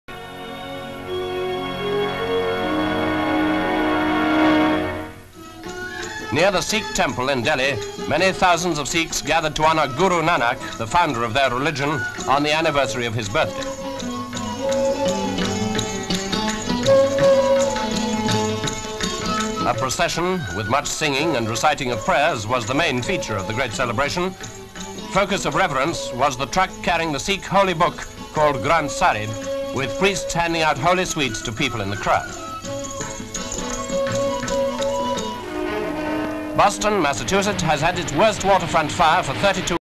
NEWS BRIEFS - SIKH FESTIVAL
(10 Nov 1952) Near the Sikh Temple in Delhi, many thousands of Sikhs gathered to honour Guru Nanak, the founder of their religion, on the anniversary of his birthday.